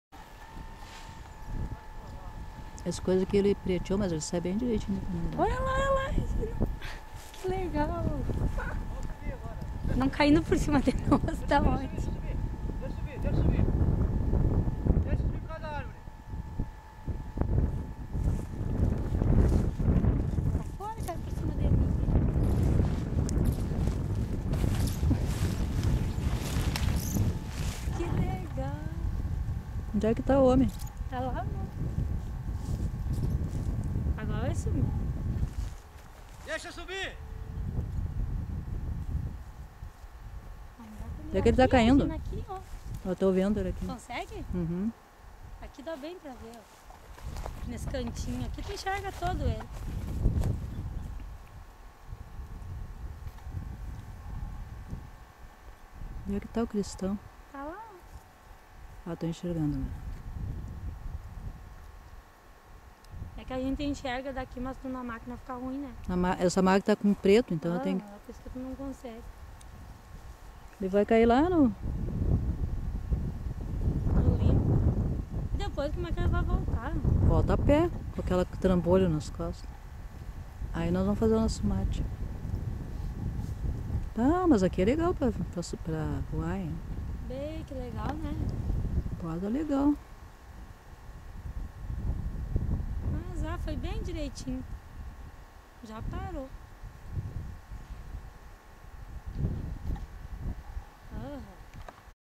PARAPENTE NO MORRO DO BISCAI PILOTO FLAVIO MAIOLLI
DILERMANDO DE AGUIAR - RS - BRASIL , MORRO BISCAI VOO LIVRE DE PARAPENTE